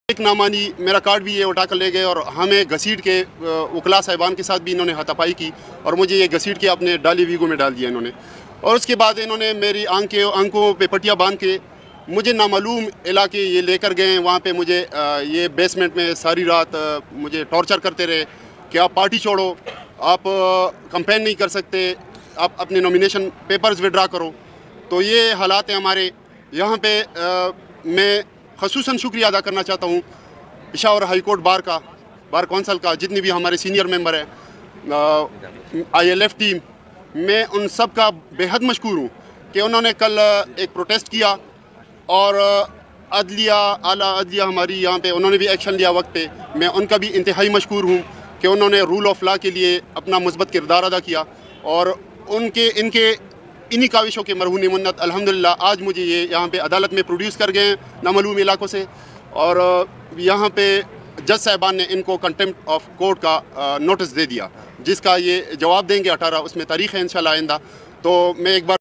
‏پشاور ہائیکورٹ نے آفتاب عالم کو رہا کردیاکل رات مجھے نامعلوم جگہ لے گئے جہاں مجھ ہر تشدد کرتے رہے
‏پشاور ہائیکورٹ نے آفتاب عالم کو رہا کردیا 🚨🚨🚨
کل رات مجھے نامعلوم جگہ لے گئے جہاں مجھ ہر تشدد کرتے رہے کہ پی ٹی آئی چھوڑ دوں۔۔۔۔۔